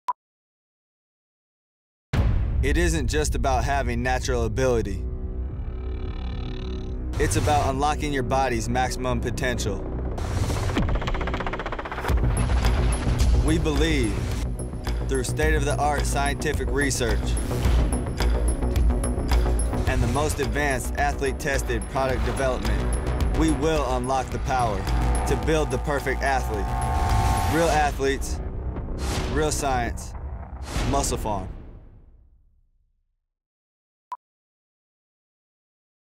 MusclePharm® | Real Athletes. Real Science. Feat. Colin Kaepernick 30sec
Colin Kaepernick started on a youth football team at age 8 and was starting quarterback within a year, foreshadowing a career of unstoppable dominance on the field. After an award-winning college career at the University of Nevada, Kaepernick was drafted second round, 36th overall pick in the in the 2011 draft. In his first playoff game, Kaepernick set an NFL record for most rushing yards by a quarterback in a playoff or regular season game. This year he lead the 49ers to the Super Bowl as NFC champions. Being in absolute peak condition is his way off life, and we are proud to have Colin Kaepernick as a part of the MusclePharm team.
Colin Kaepernick